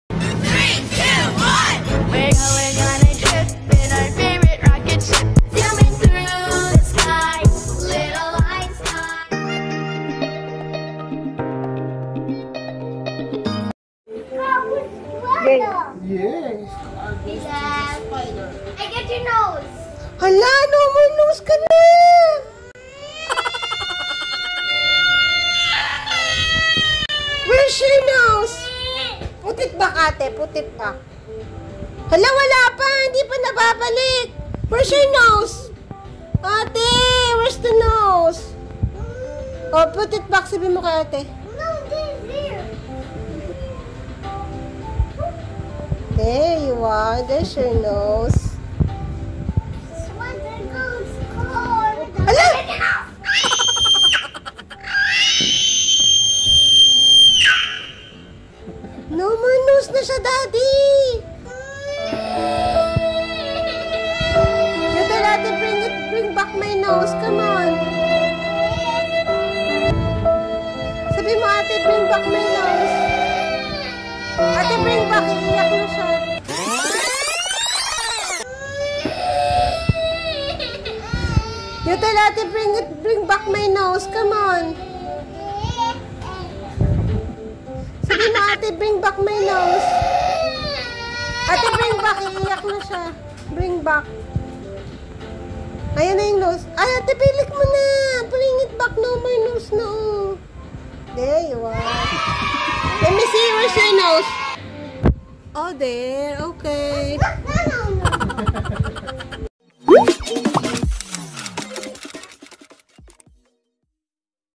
Pranking Kaleb part 2 - Her sister stealing his Nose🤣
Welcome to Kaleb Show!

PRANK TIME: big sister and mommy pranking baby KaleB, funny reaction from Kaleb😂
scenario: big sister steal his baby brother's  nose 😅
VICTIM: Kaleb
ACCOMPLISH: Ate Kathlene, Mommy and Daddy